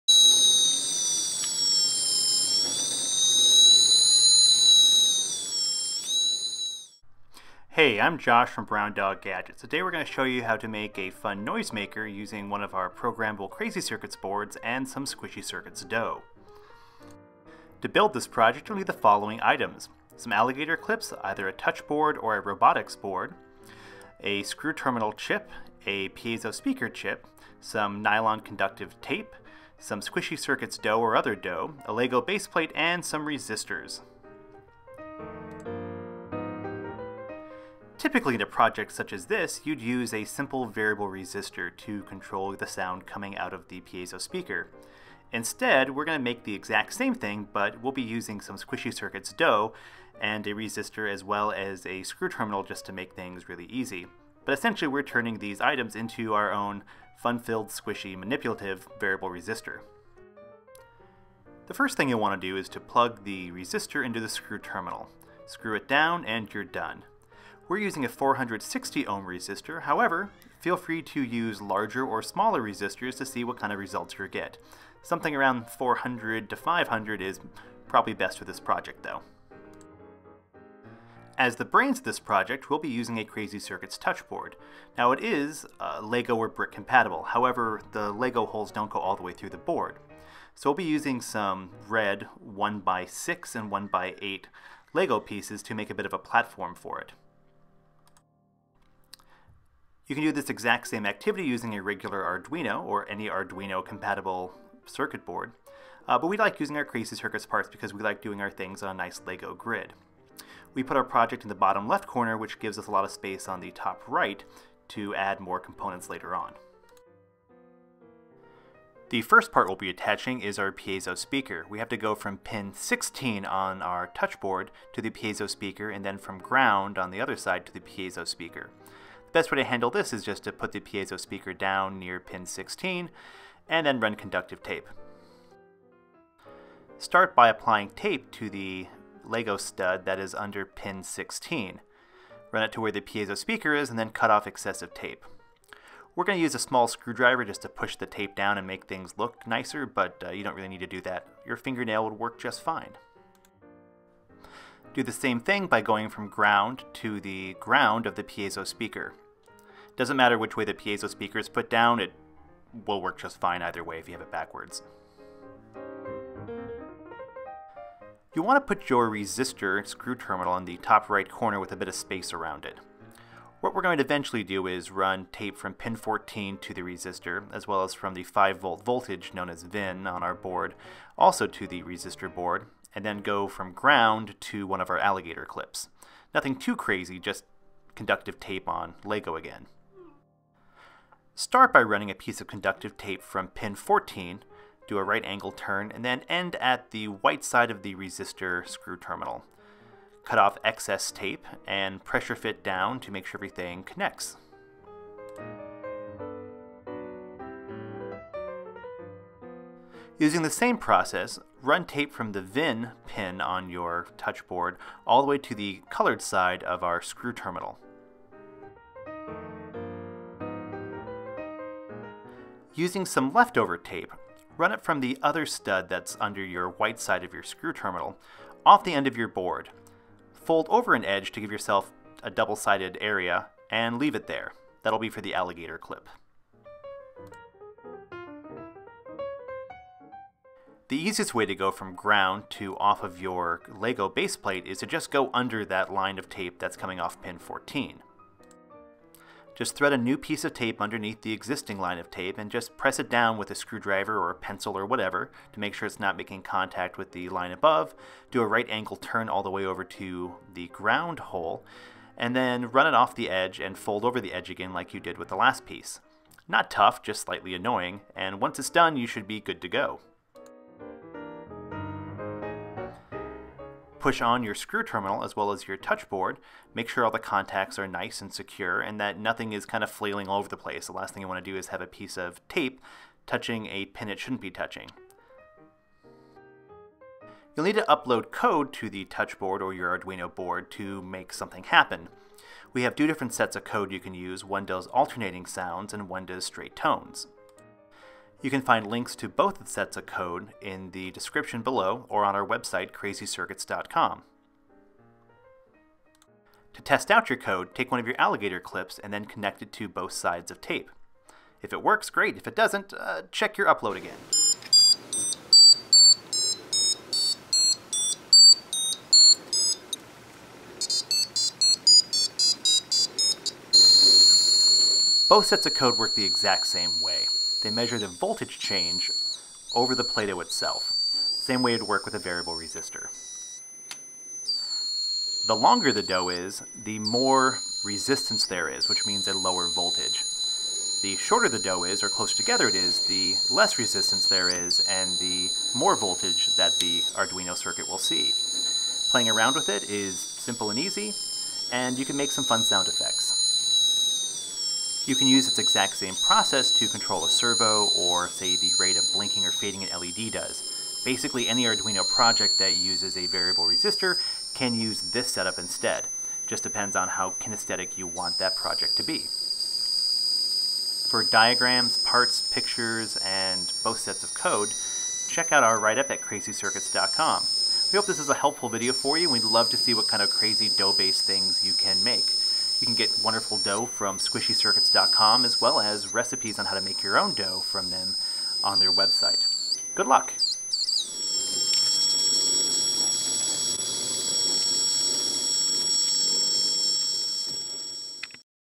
Squishy Circuits Sound Effects
Create fun sound effects using Squishy Circuits Dough and Crazy Circuits parts. It's simple and easy to do. Check the links below for step by step directions as well as the code we used on this project.

Grab a kit from CrazyCircuits.com.